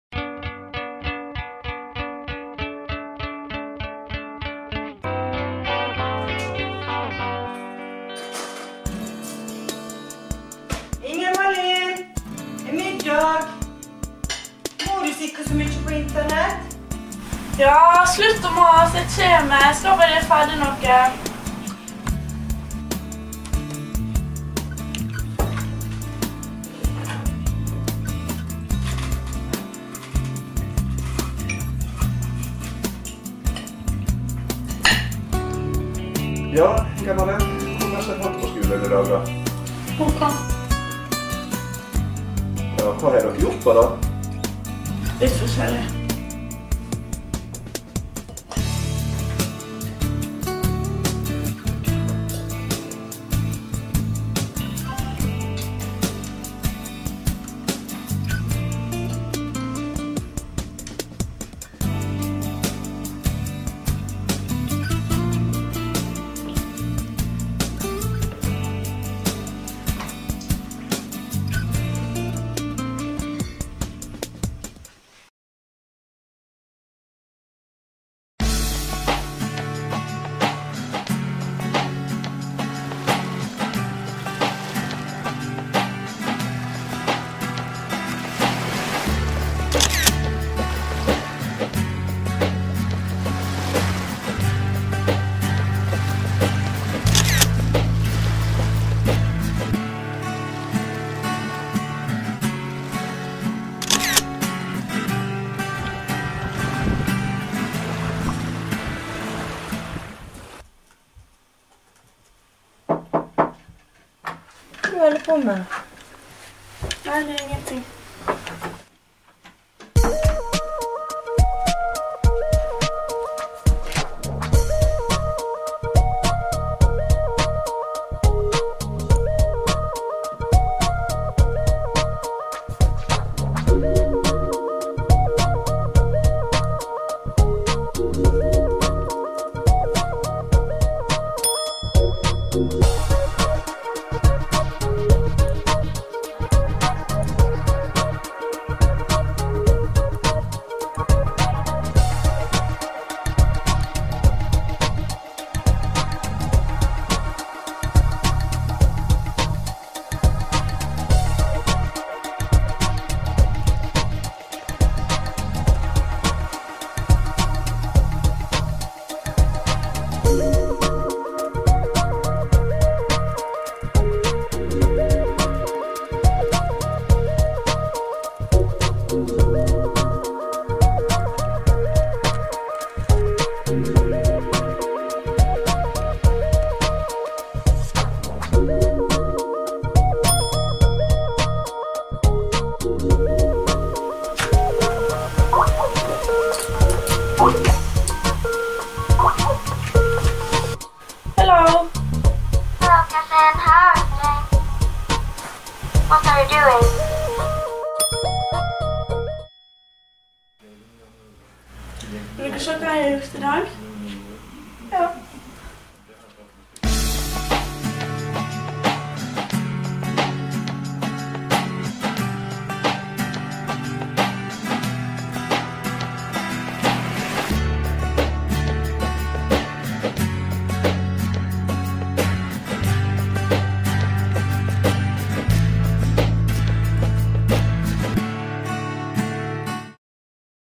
Sosial Web
Film om Sosial Web og Kunnskapsløftet. Eksamensoppgåve våren 2010 for gruppe 2 på DKL104.